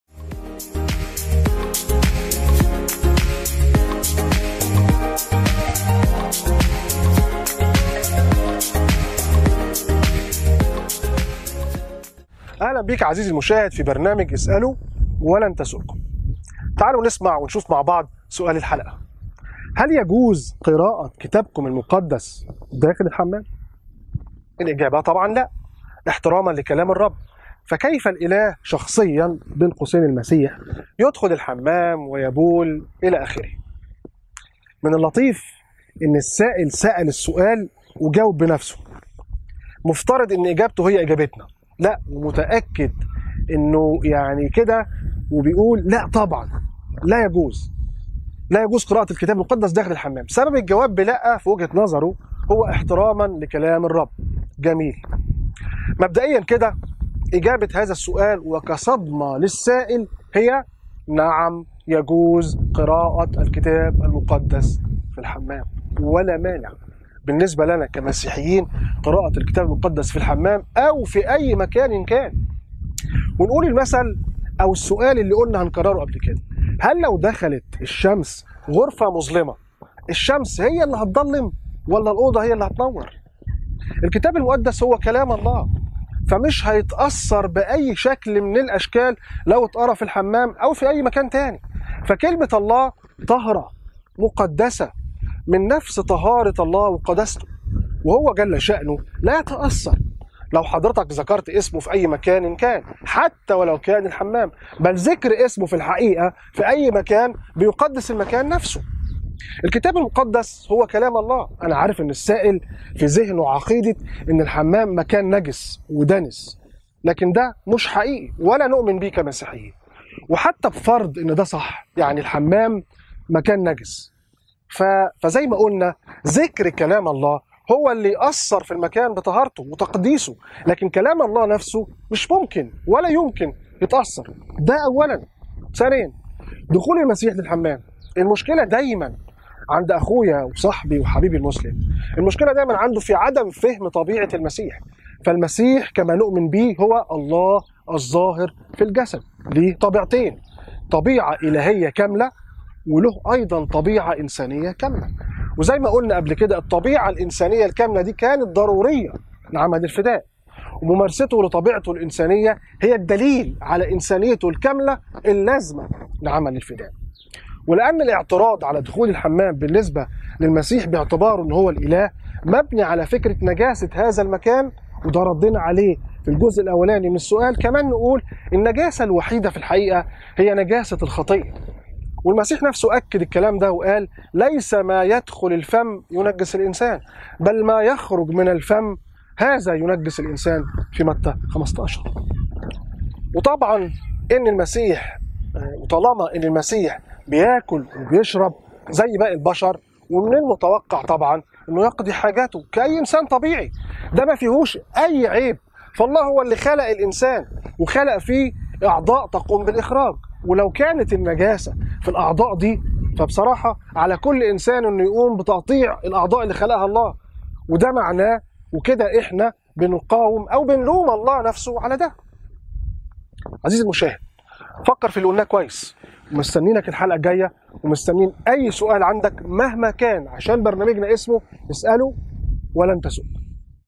اسألوا ولن تسؤكم |22| كتاب الله في الحمام
هل يتأثر كتاب الله أو كلامه عند قراءته في الحمام، ولماذا الحمام نجس، وهل يستطيع تنجيس كتاب الله أو كلامه؟ إن دخلت الشمس غرفة مظلمة، هل الشمس ستظلم أم الغرفة ستنير؟